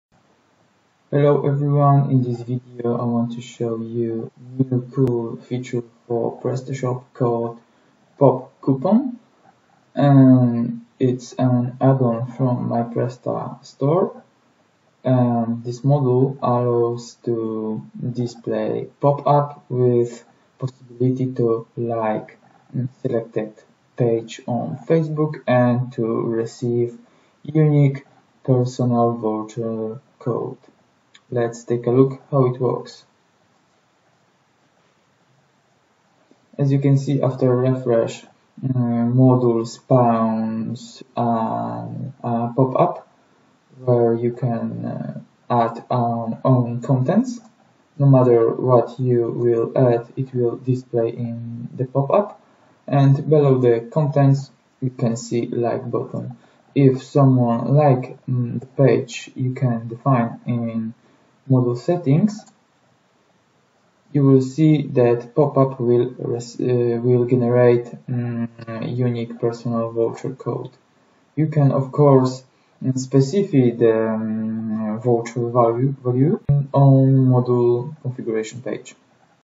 Responsive pop coupons for PrestaShop
This PrestaShop module allows to display unlimited number of popups with special button to like page you define (your shop page, fanpage or facebook etc.). Customers for like will receive special coupon code (unique, generated only for them)